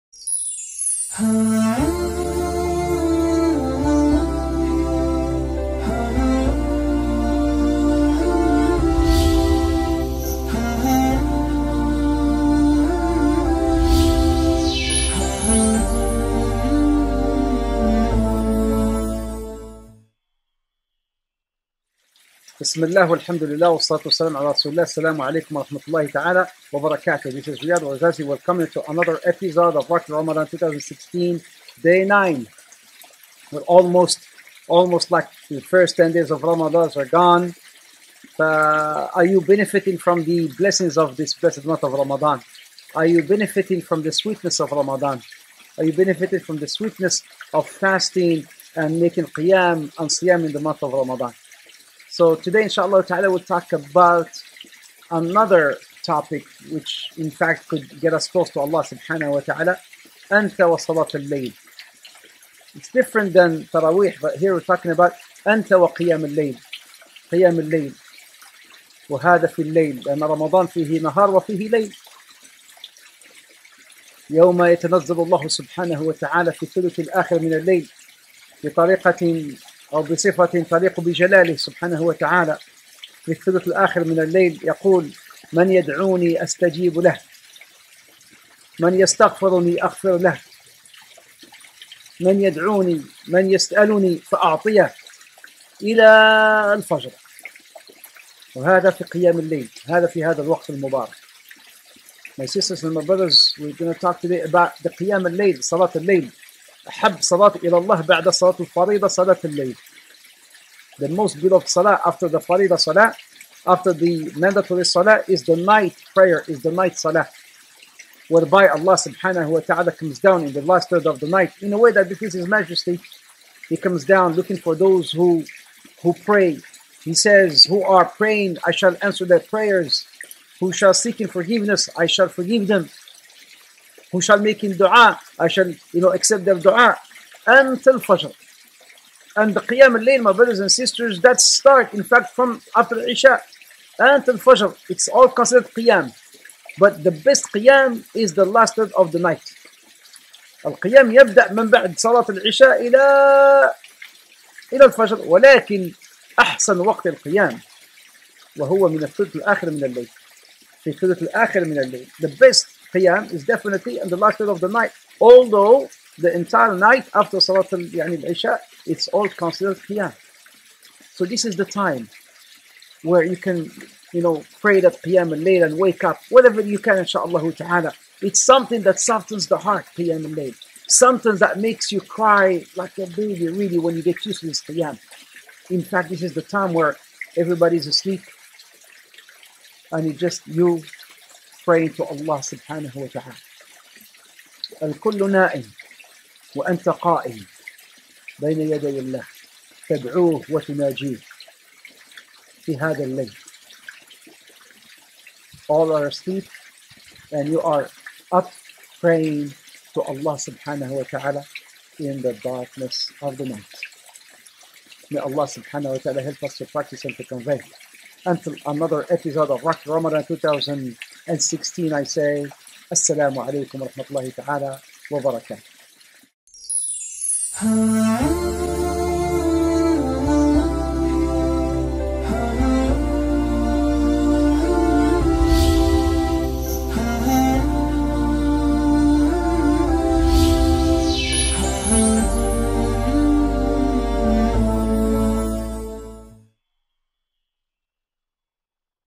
Roc Your Ramadan 2016 : Day 9 - Blessings of Qiyaam ul Layl - the night prayer
Day 9 -Blessings of Qiyaam ul Layl - the night prayer - Ramadan Mubarak - ROC your Ramadan 2016, a daily video series covering the virtues of Ramadan by Shaykh Riad Ouarzazi.

For more information on future events, seminars, or upcoming Roc retreats with Shaykh Riad Ouarzazi please